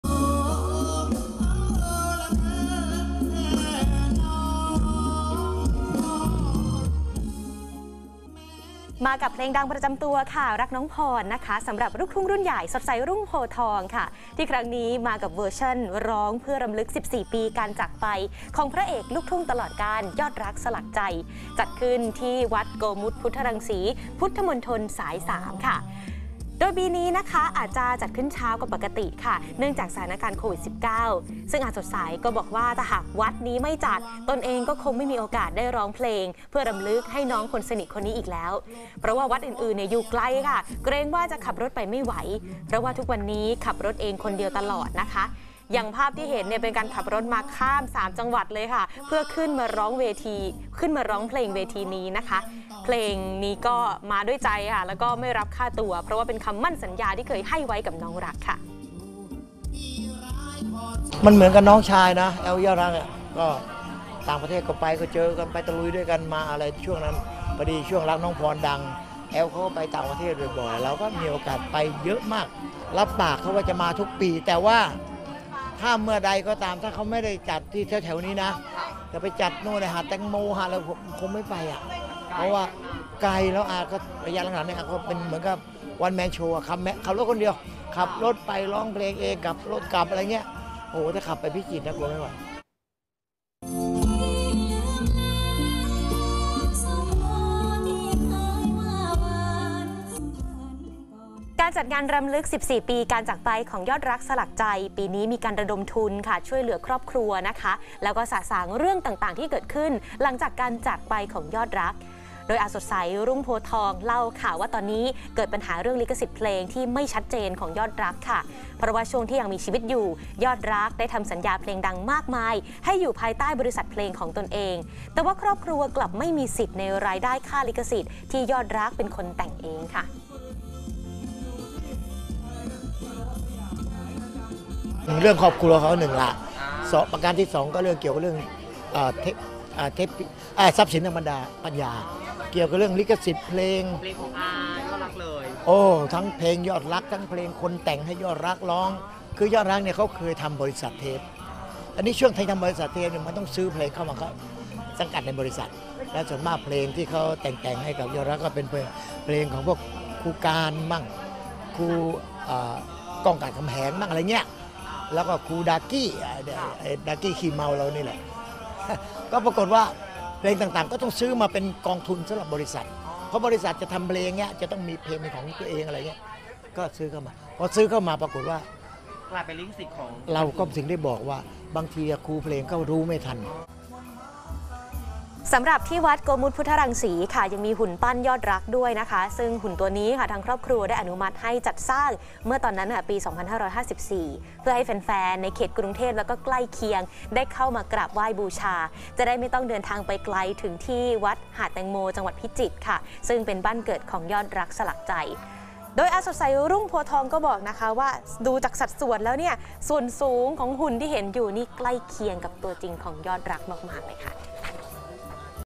"สดใส รุ่งโพธิ์ทอง" ร้องเพลงรำลึก "ยอดรัก สลักใจ"
เป็นประจำทุกปีที่วัดโกมุทพุทธรังสี จะจัดคอนเสิร์ตรำลึกการจากไปของพระเอกลูกทุ่งตลอดกาล "ยอดรัก สลักใจ" และเป็นประจำทุกปีที่รุ่นพี่อย่าง "สดใส รุ่งโพธิ์ทอง" จะเดินทางมาร่วมร้องเพลงเพื่อรำลึกถึงน้องชายสุดที่รักในวงการ โดยการจัดงานครั้งนี้ ยังมีการเชิญชวนแฟนเพลงร่วมสมทบทุน เพื่อนำเงินไปช่วยเหลือครอบครัวของยอดรัก โดยเฉพาะปัญหาลิขสิทธิ์ที่เกิดขึ้นหลังนักร้องดังเสียชีวิต